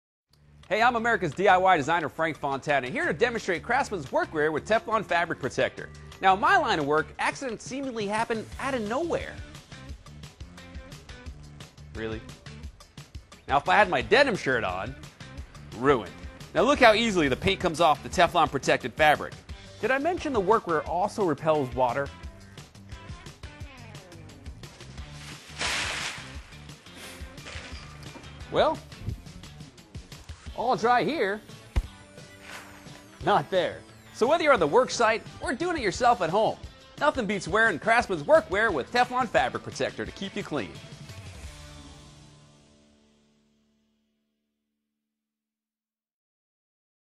Get Into Gear with Craftsman Work Wear with Teflon® Fabric Protector [2/3]
Frank Fontana Wears Craftsman Work Wear with Teflon® Fabric Protector